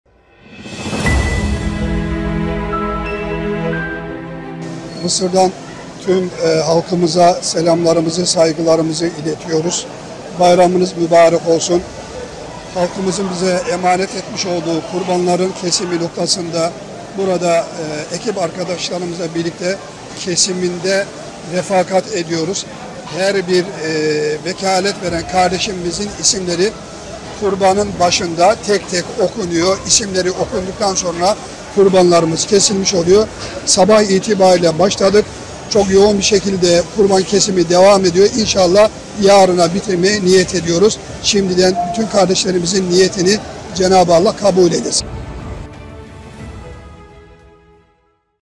Filistinli kardeşlerimiz için kurbanlıkları Mısır'da hazırlıyoruz
Soykırımla karşı karşıya olan Filistinli kardeşlerimize milletimizin kurban emaneti ulaştırmak için Mısır'da görevli ve gönüllülerimiz canla başla hazırlıklarını sürdürüyor.

KurbanınıPaylaş Kardeşinle Yakınlaş